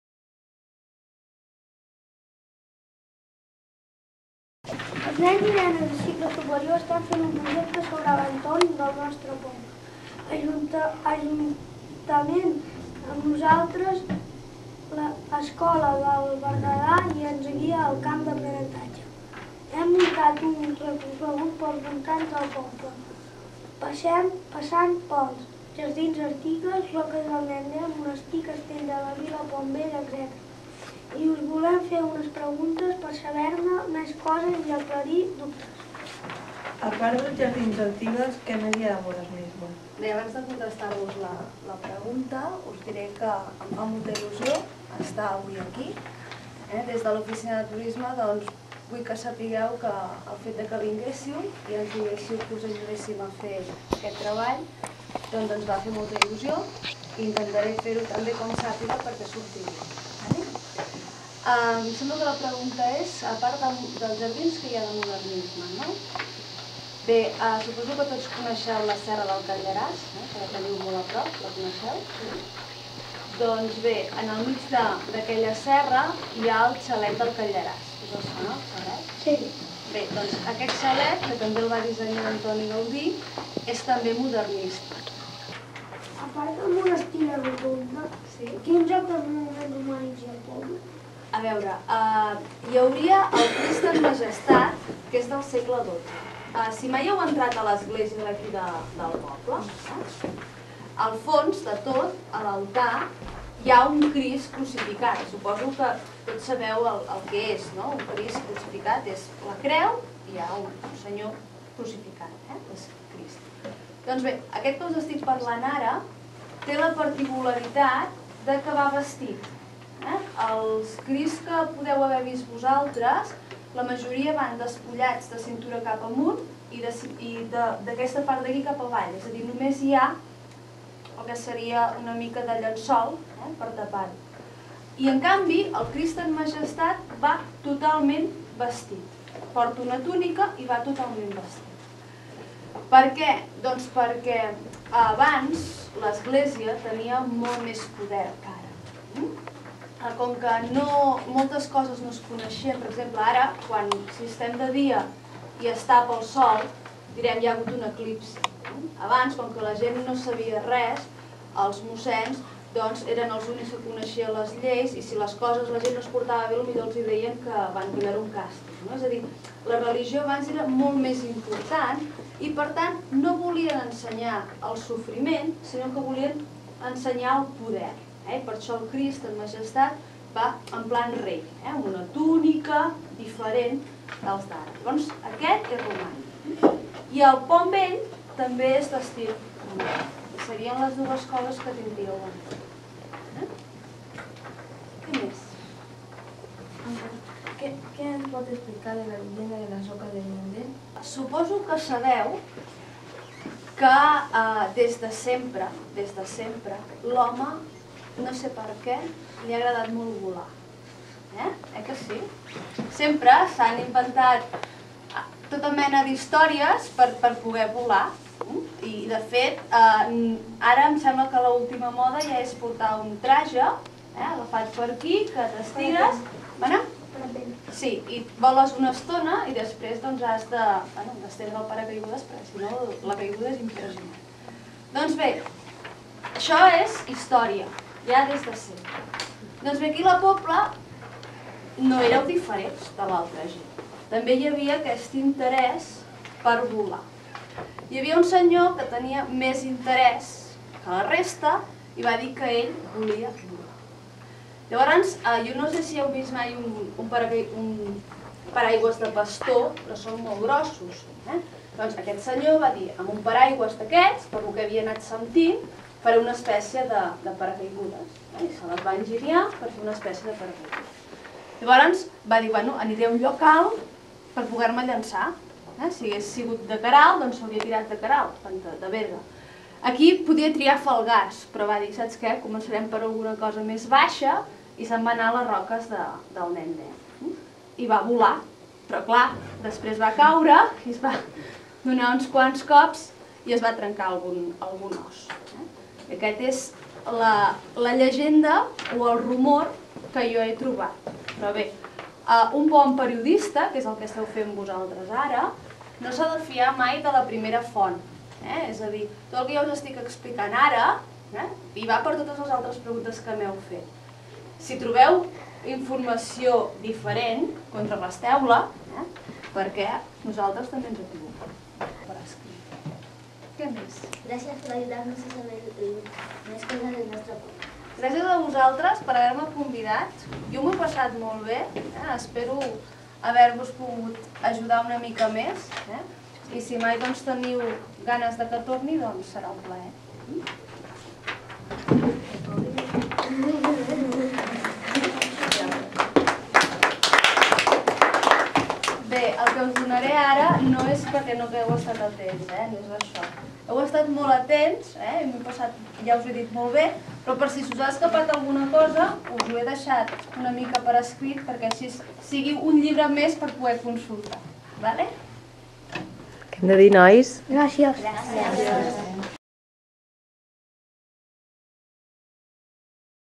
Entrevista
Els Alumnes de Cicle Superior del CEIP Lillet a Güell han fet una entrevista a la Gemma de l'oficina de turisme de la Pobla de Lillet